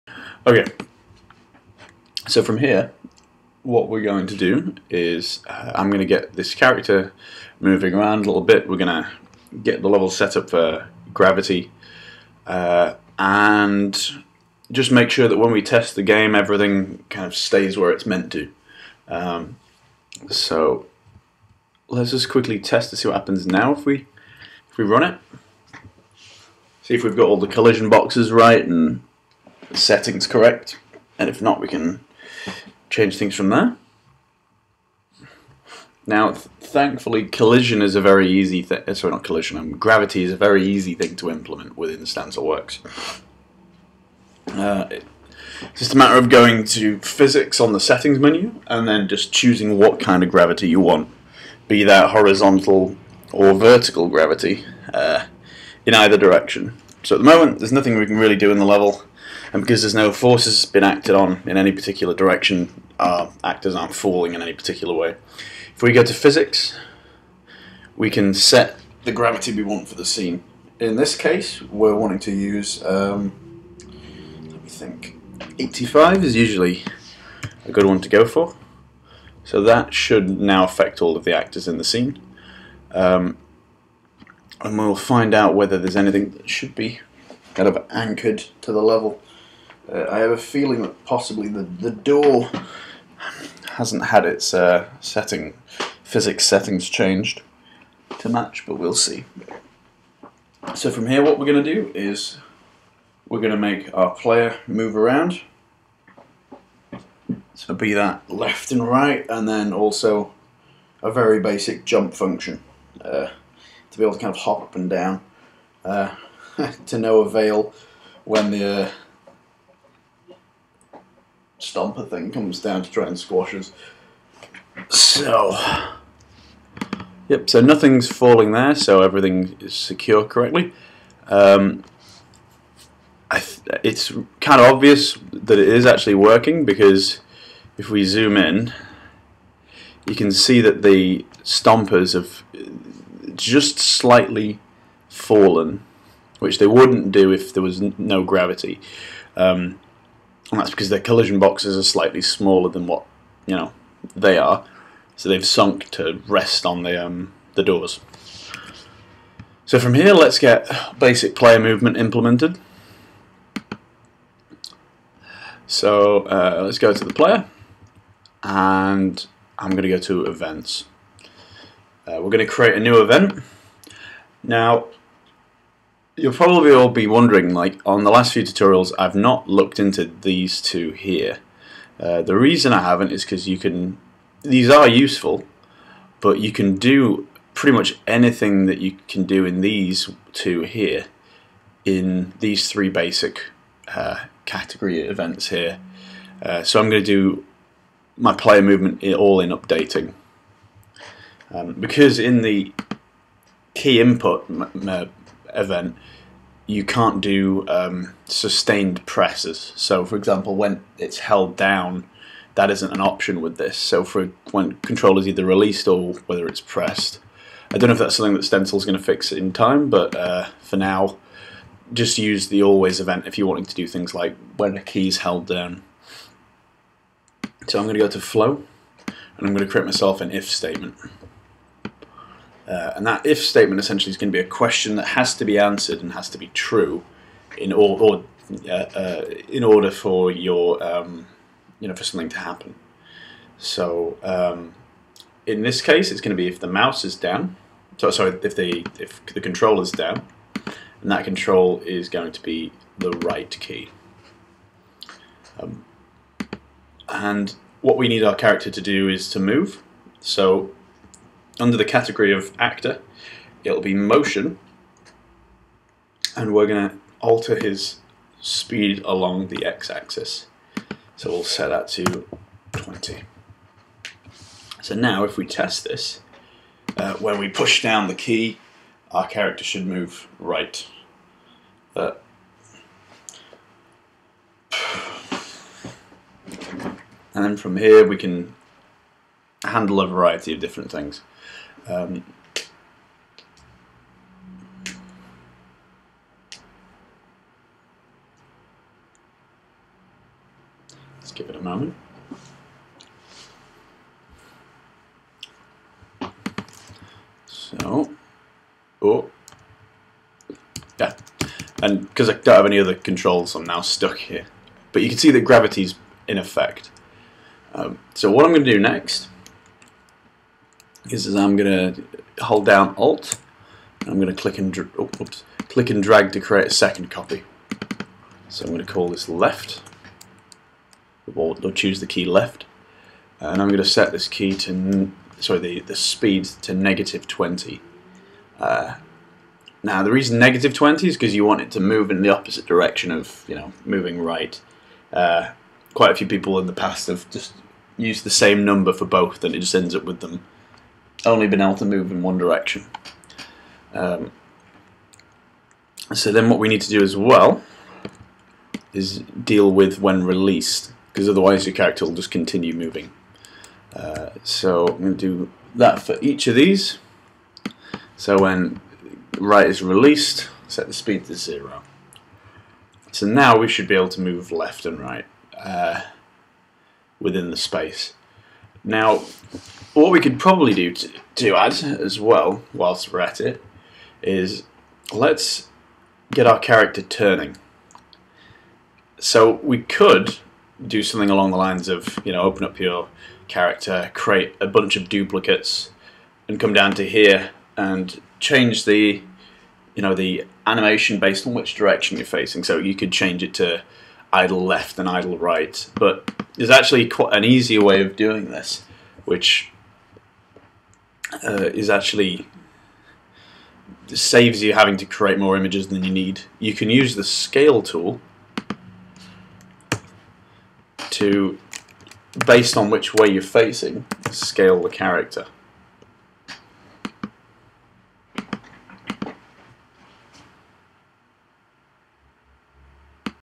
Building a Basic Platformer in Stencyl - PART 2a
In this section of the tutorial, we'll get the player moving around in the space, along with refining some of their features.